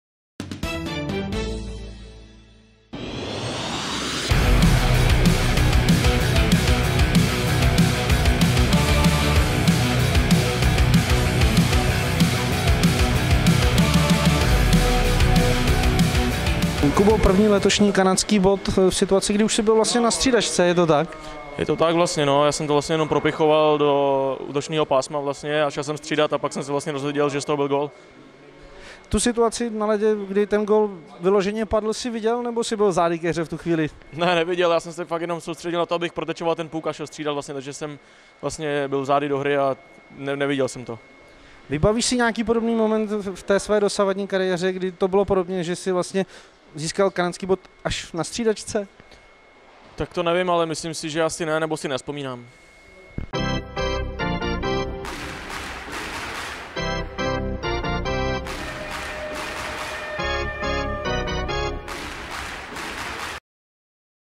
FISAD MOMENT UTKÁNÍ 41. kolo TELH: První bod Jakuba Chrpy
Ve 41. kole naskočil v pirátském dresu po šesté v letošním ročníku Tipsport extraligy a poprvé bodoval. Útočník Jakub Chrpa si připsal první asistenci u jediného gólu, který vstřelil jeho jmenovec a navrátilec do sestavy Sklenář. Kuriozitou celé situace je, že v době, kdy puk přešel brankovou čáru, už byl chomutovský odchovanec na střídačce, takže si do statistik nezapsal pozitivní účast. Po závěrečné siréně čtyřiadvacetiletý Jakub Chrpa marně lovil v paměti, jestli už něco podobného zažil.